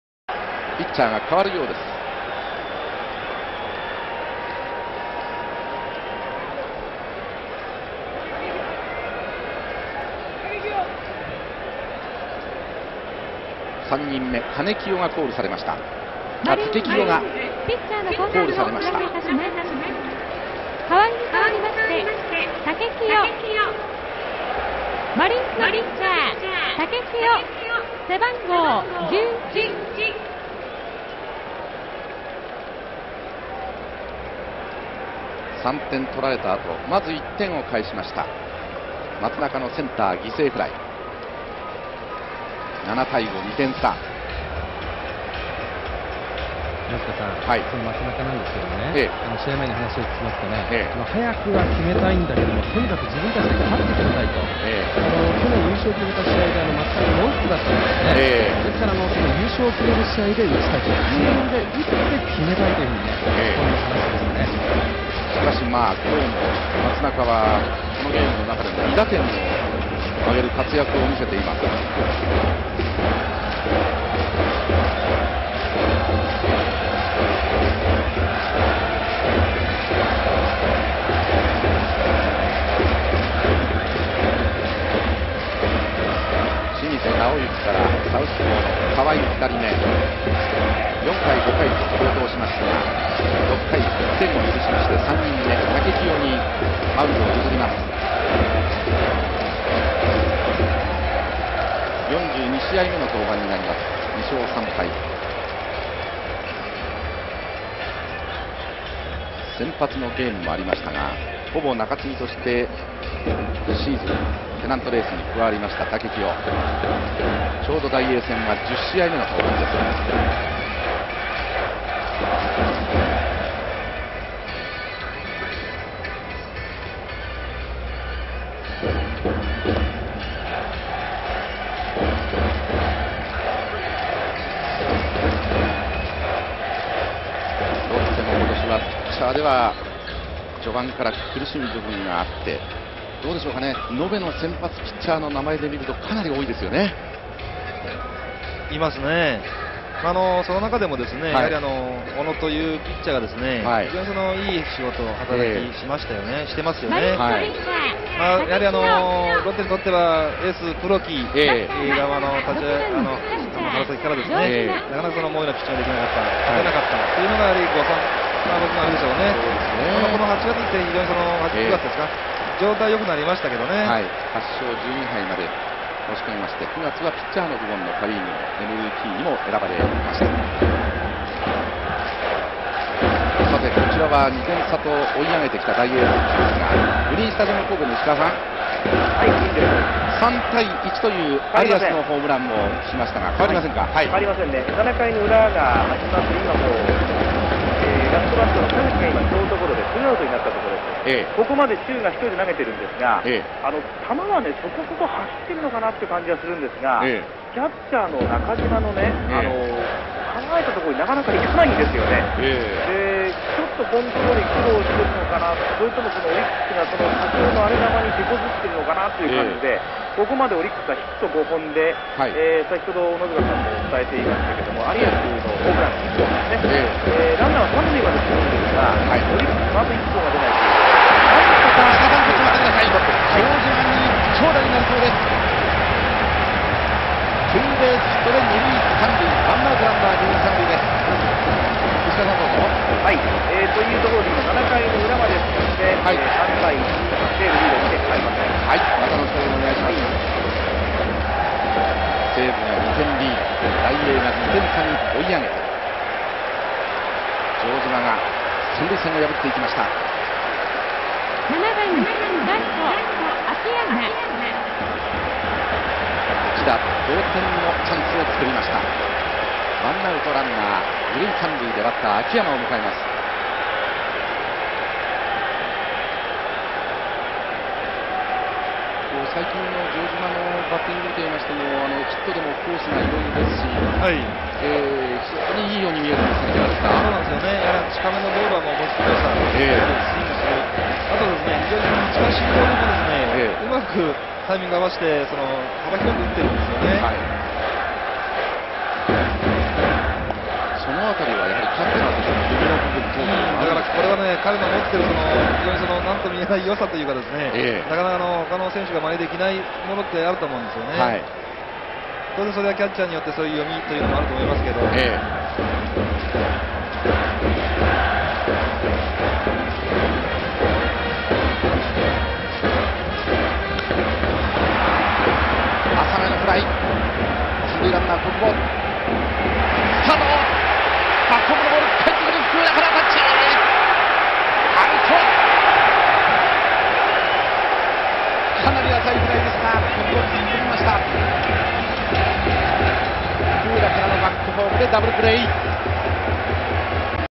【千葉ロッテマリーンズ】竹清 投手
96年ドラフト２位で入団。即戦力として背番号11を与えられ、ルーキーイヤーは春先から一軍に定着したが3勝10敗と散々な成績だった。00年には敗戦処理や谷間の先発などで44試合に登板、02年まで在籍。(個人的には、このシーンのおかげで福浦は強肩だというイメージが未だに残っている)